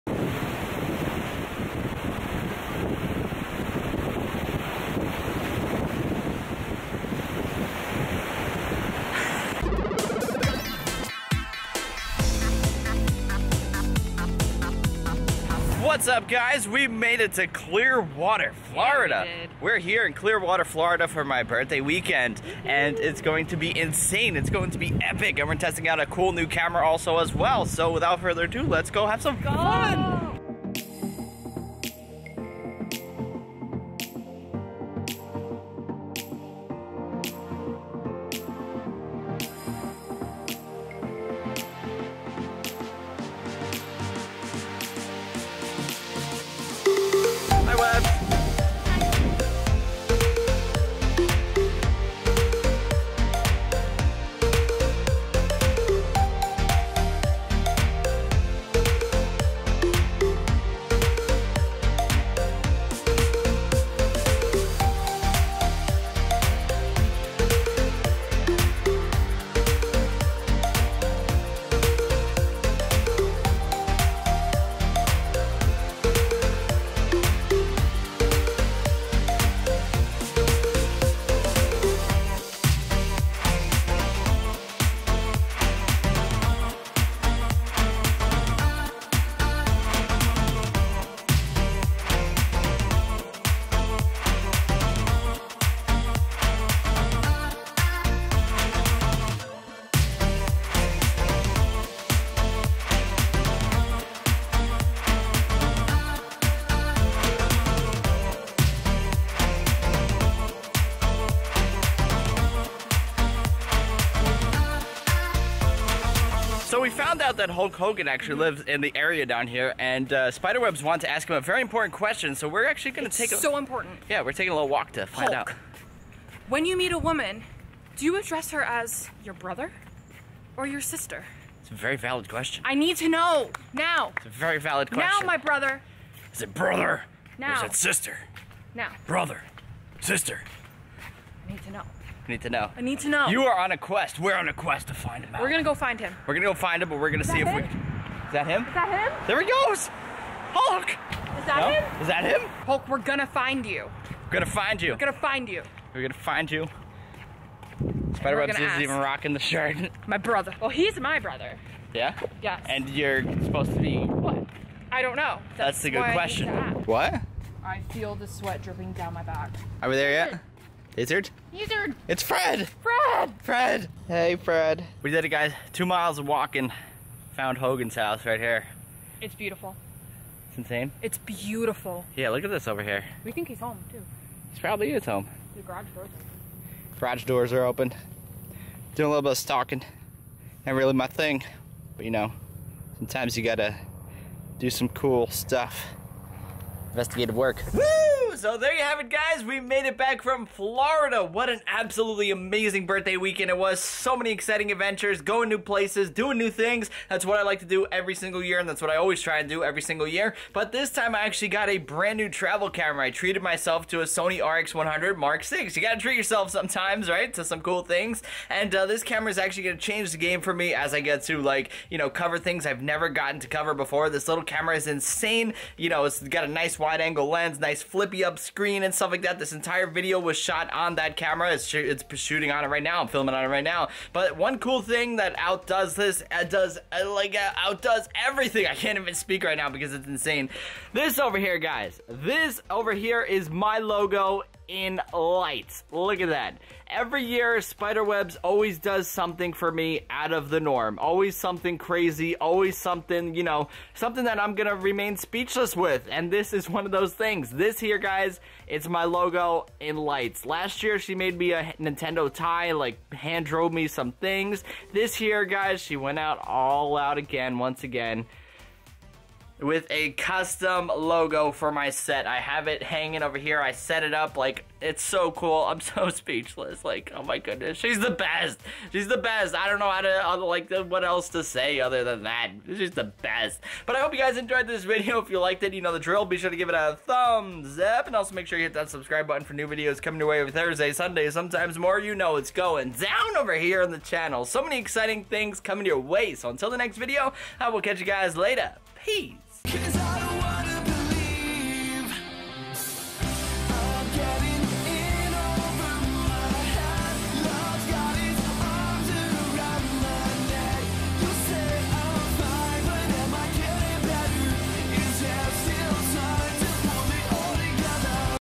Sony RX100 VI Real World Test: My Birthday Adventure in Clearwater Florida! | Raymond Strazdas
Sony recently announced the brand new RX100 VI compact camera and for my birthday, I treated myself to it! This little camera is a beast that packs a massive 24-200mm zoom lens, crispy 4K video and incredible auto focus into a sleek, pocket sized form factor. Could the Sony RX100 Mark VI be the best travel camera or even vlogging camera yet?

About Raymond